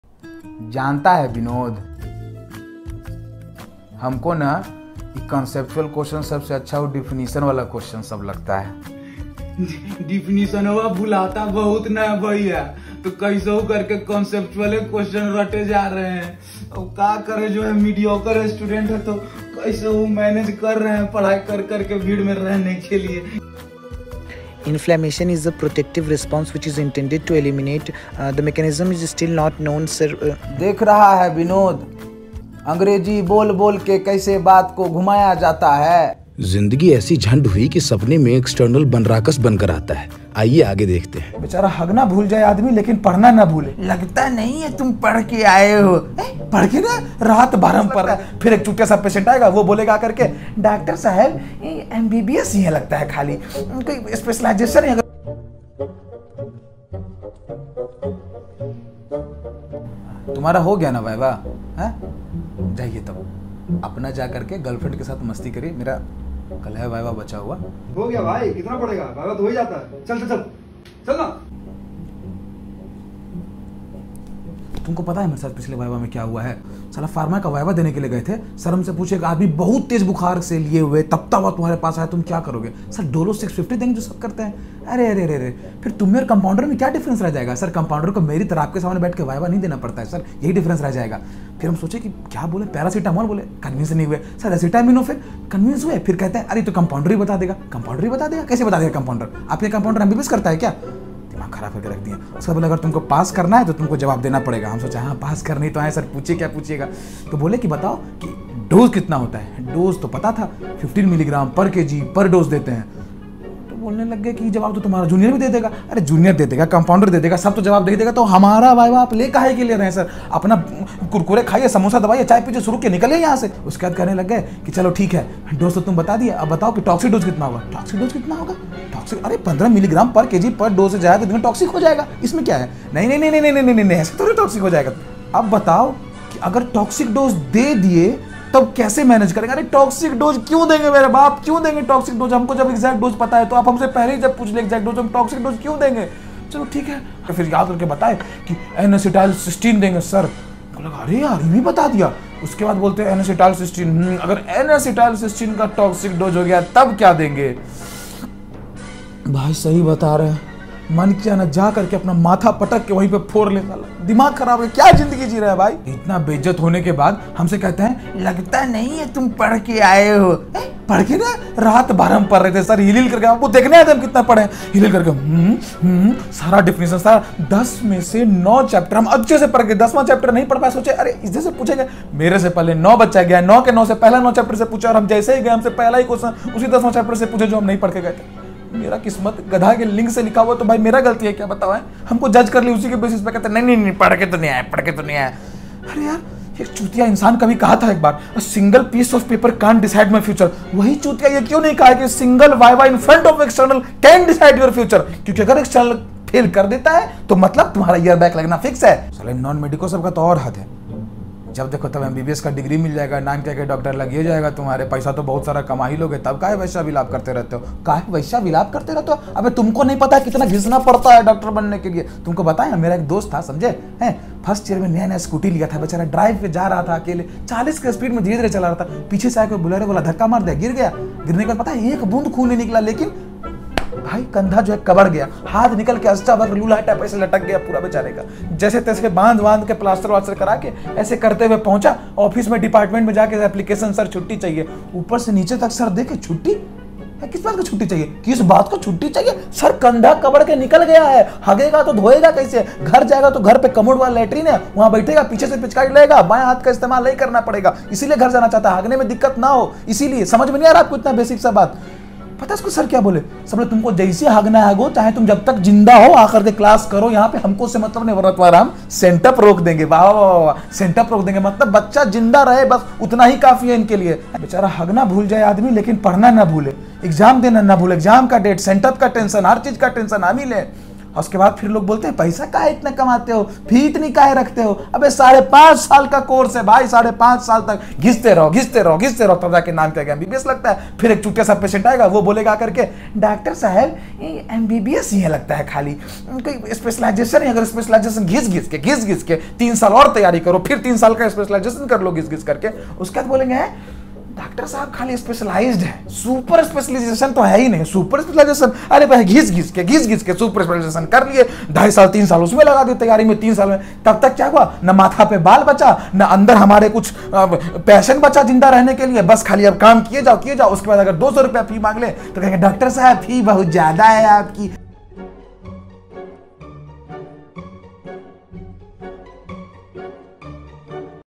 BACK-BENCHER MBBS NIBBA | must watch before MBBS
Back bencher Binod and angry professor banraakas
Frustrated MBBS student who is a backbencher .
Video is made just for fun